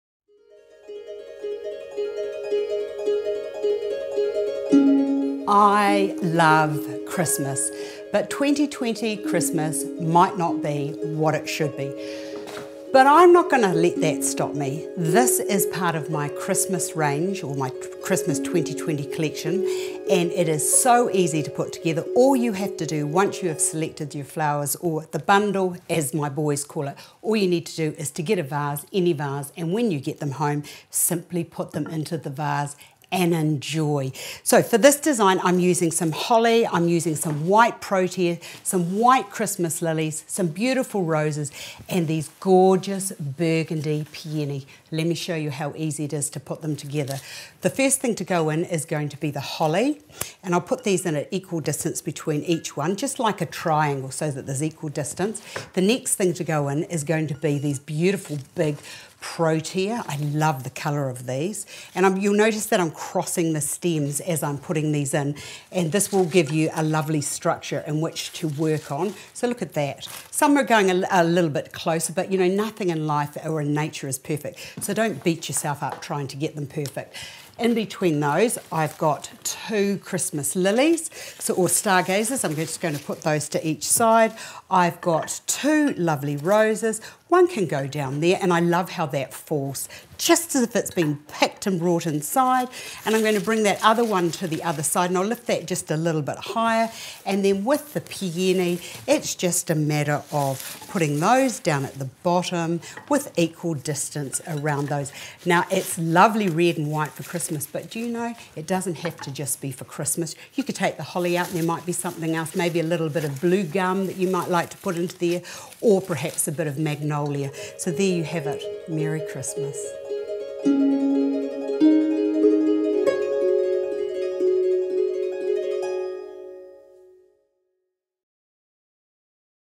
Flowers in a moment: Winter whites and plum wines
How is this stunning combination of heavenly white frosted protea, plum wine peony, blown roses and green laden holly berry, all gathered to lift you gently into the spirit of Christmas? My favourite in the collection!

Collection consists of:
- 2 white Christmas lilies
- 2 red roses
- 3 green holly and/or 3 red holly
- 3 white protea
- 2 burgundy red peonies

You will also need at vase, around 12” is good!

Happy Holidays and may the Christmas Spirit fill you with good cheer!